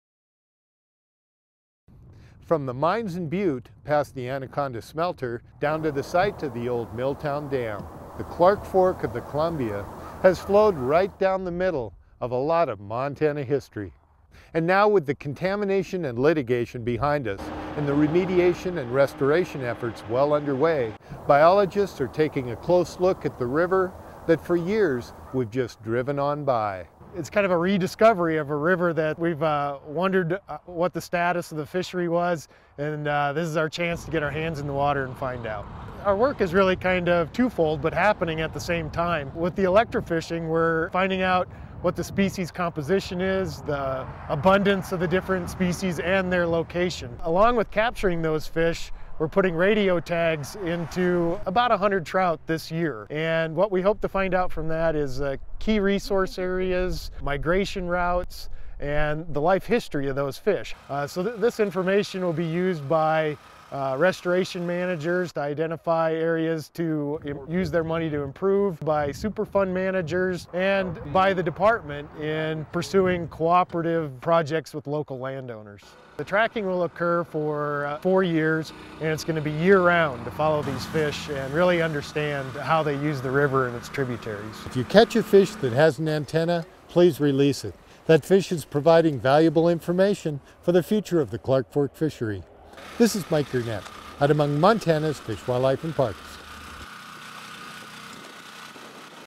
Clark Fork River Comeback
From the mines in Butte to the site of the old Milltown Dam, the Clark Fork of the Columbia has flowed down the middle of a lot of Montana history.  And now, the with the contamination and litigation behind us, it's being restored to it former glory.